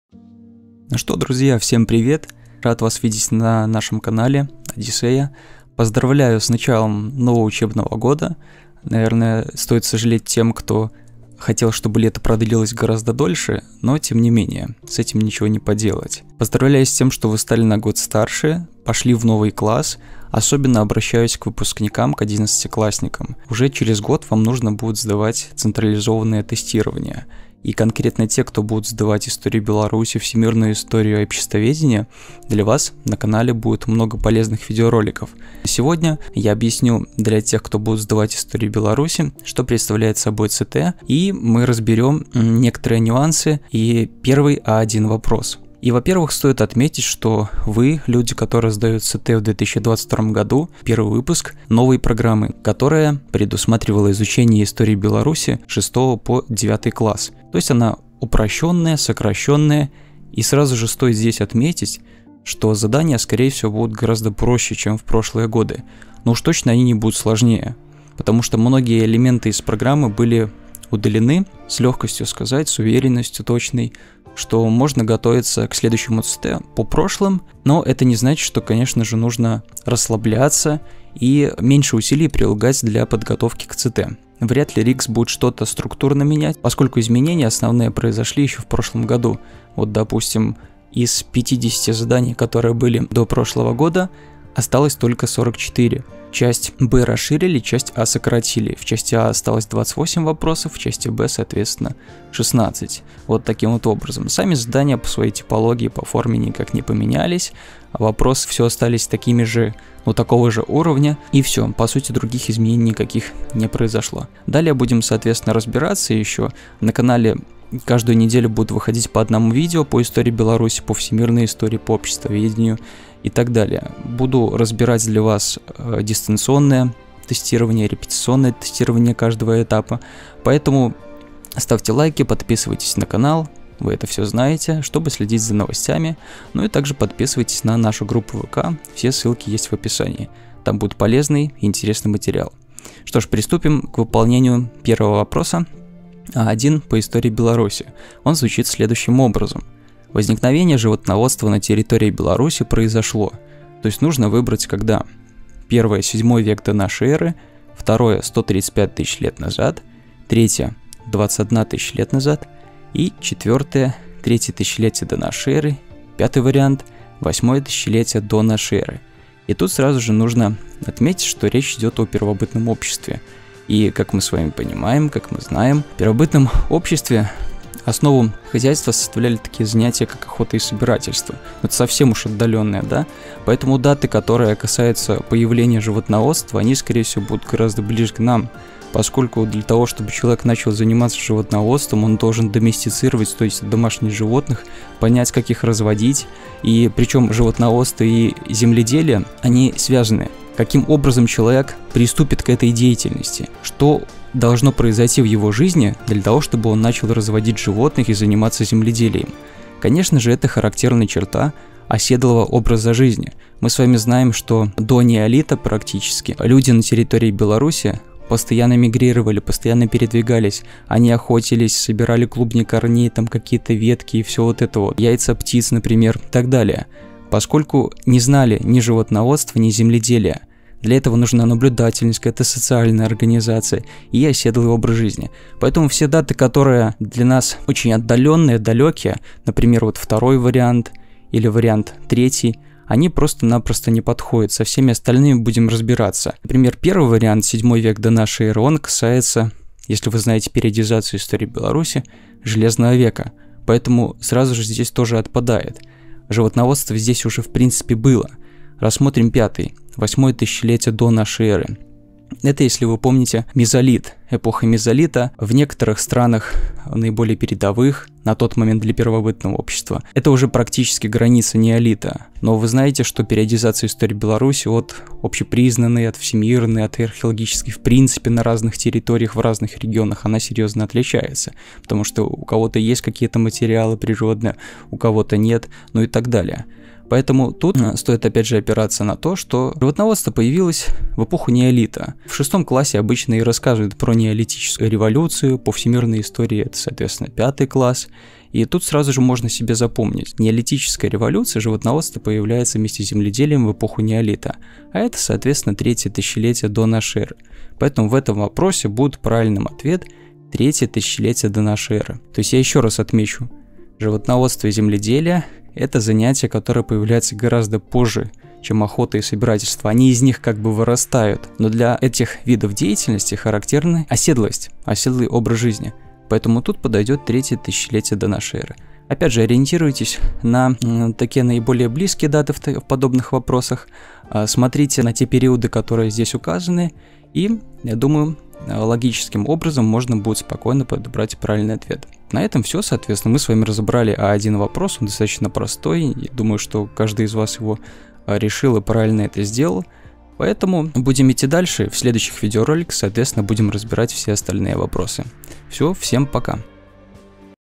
История Беларуси. ЦТ: А1
На этом уроке мы расскажем вам о ключевых событиях истории Беларуси, которые вам необходимо знать для сдачи ЦТ. Вы узнаете о важных исторических личностях, периодах развития страны и его влиянии на современность.

Мы предоставим вам полезные материалы для подготовки, советы по эффективному запоминанию информации и методики ответов на типовые вопросы ЦТ.